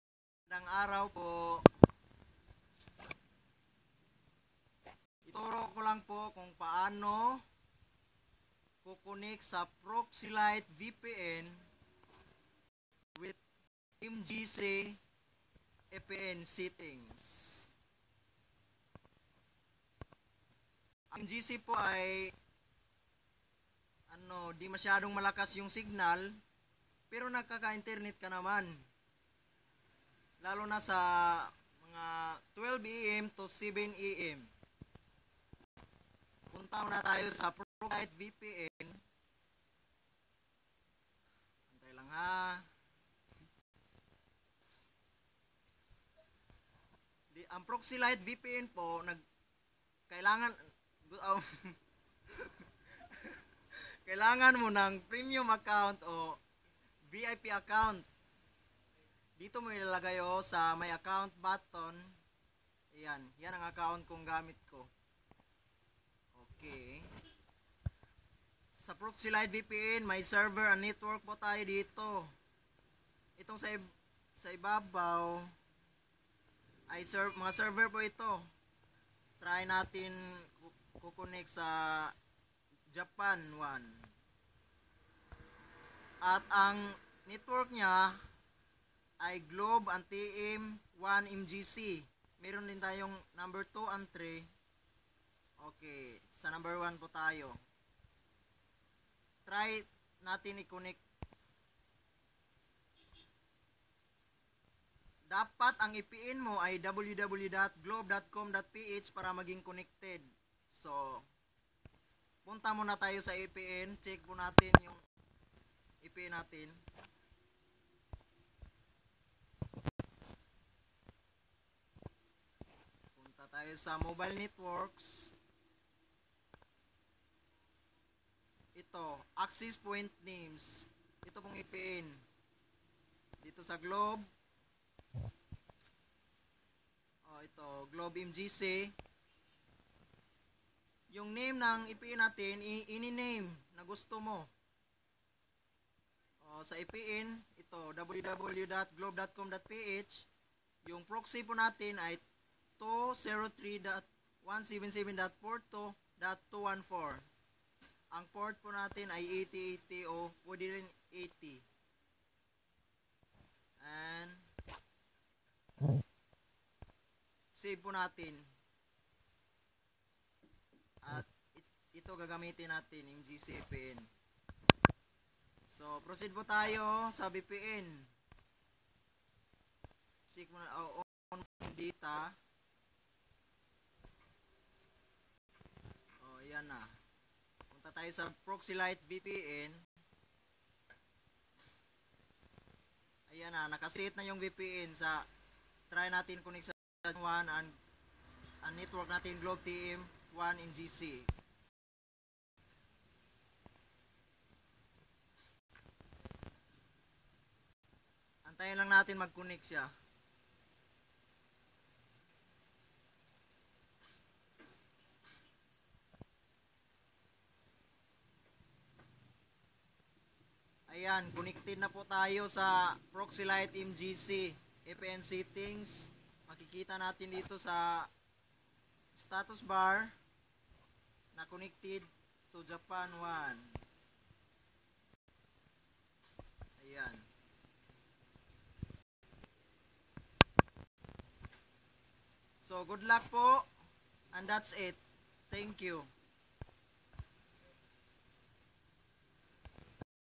How to Connect Proxylite VPN (MGC-APN)
Ito na po. . .ang tutorial Kung paano po tayo mka Connect sa VPN   GAMIT ANG (MGC) APN